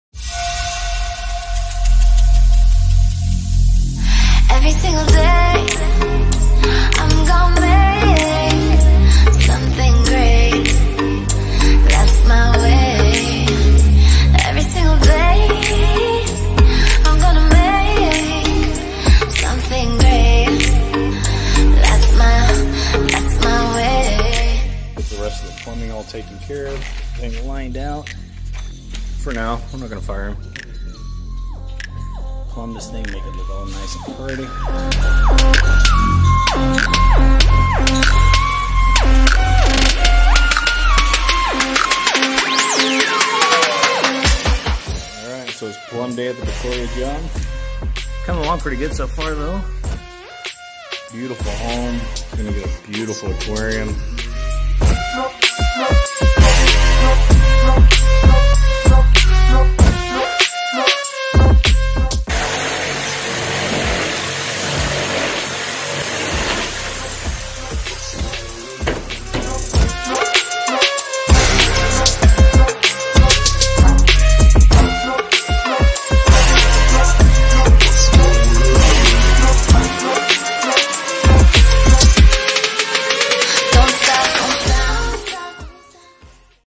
400 gallon custom saltwater aquarium install
400 gallon custom saltwater reef aquarium being installed in a beautiful home.